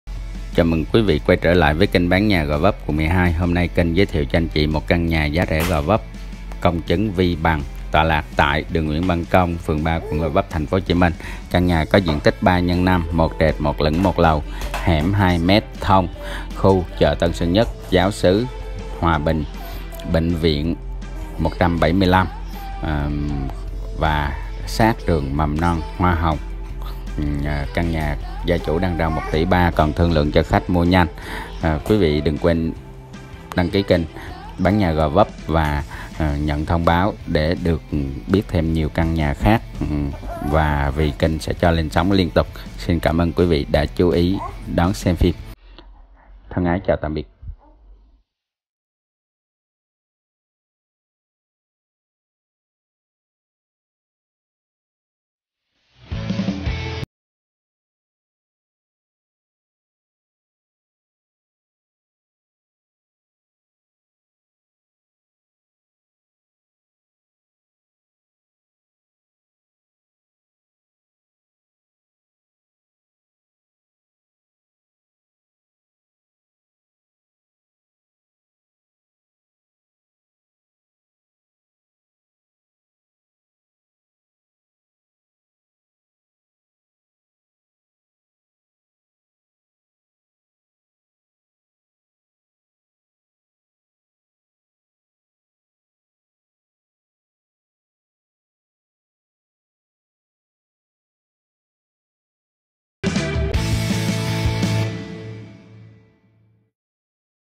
1.3 TỶ|khu Giáo xứ Hoà Bình|Nhà Bán rẻ Gò Vấp Nguyễn Văn Công, phường 3| ban nha quan go vap hom nay
1.3 TỶ|BÁN NHÀ QUẬN GÒ VẤP khu Giáo xứ Hoà Bình|Nhà Bán Giá Rẻ Gò Vấp Nguyễn Văn Công, phường 3| ban nha quan go vap hom nay
Dt 3x5m 1trệt 1lửng 1lầu hẻm 2m thông khu Mầm Non Hoa Hồng
Giá:  1.3 tỷ thương lượng CÔNG CHỨNG VI BẰNG KK99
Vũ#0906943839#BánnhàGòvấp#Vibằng
Link▶️: